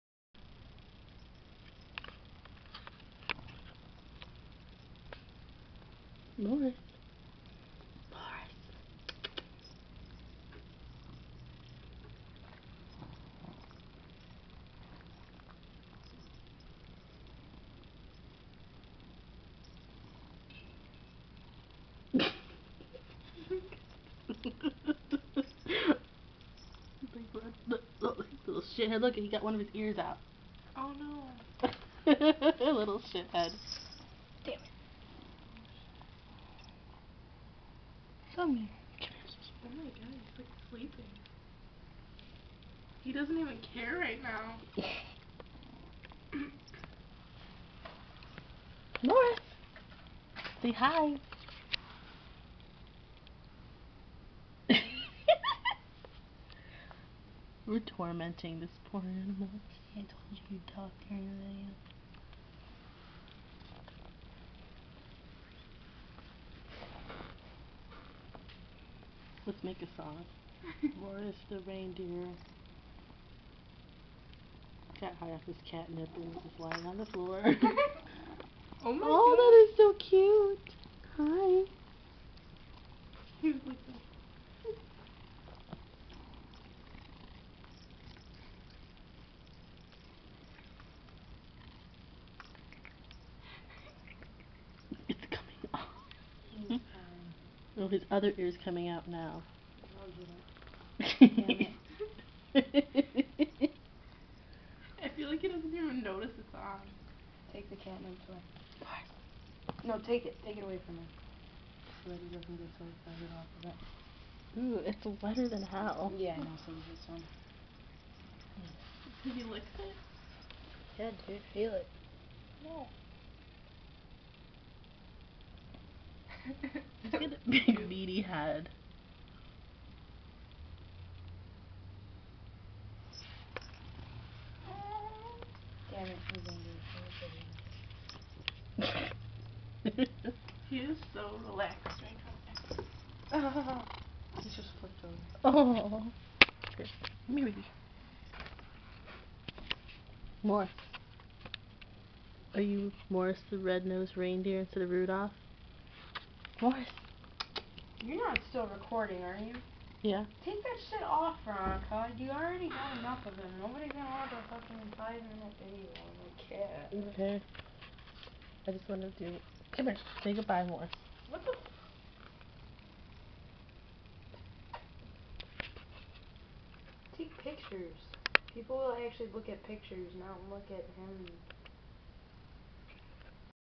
Morris the Lil' Reindeer!
My sister went to Target bought a cute lil' costume and decided to dress our pet, Morris, as Santa's reindeer. He was not having it so we gave him a toy with cat nip to coax and calm him down. He cracks me up! Excuse the cursing in the video Morris was being his crazy little self and I called him a...(you'll see what I mean). I have a feeling he will be in this for Christmas Eve and day. =P